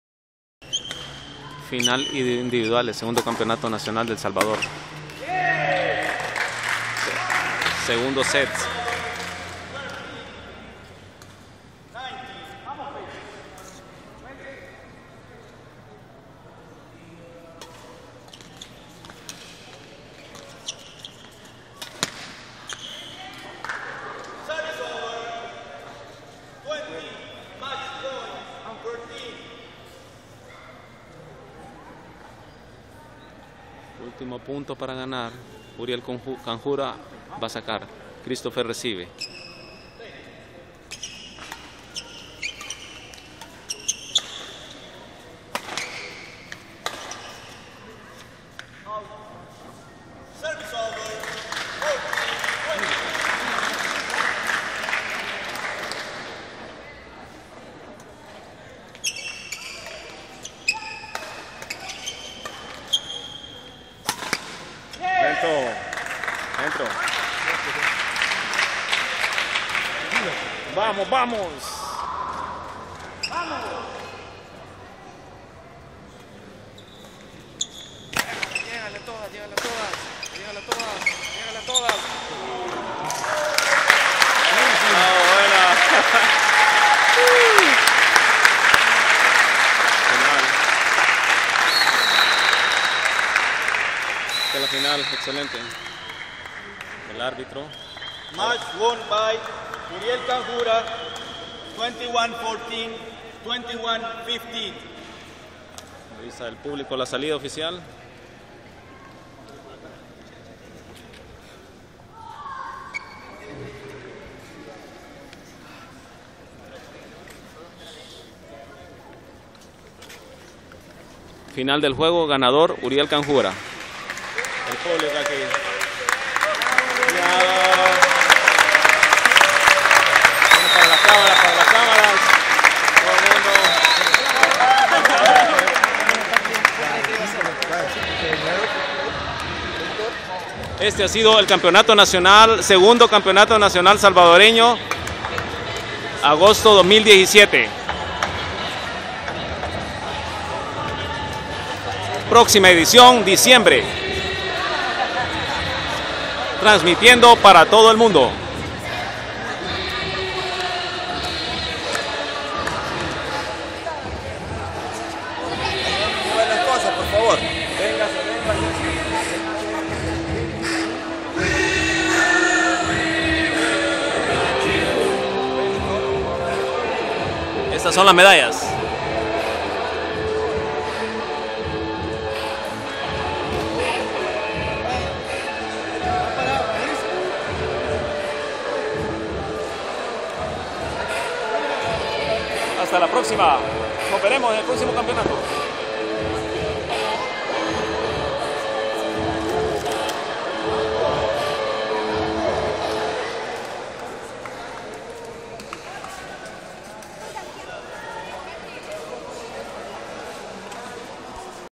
Segundo campeonato Nacional Salvadoreño de BÁDMINTON.Transmisión exclusiva de MAURI TV .
Juego Final para la medalla de oro.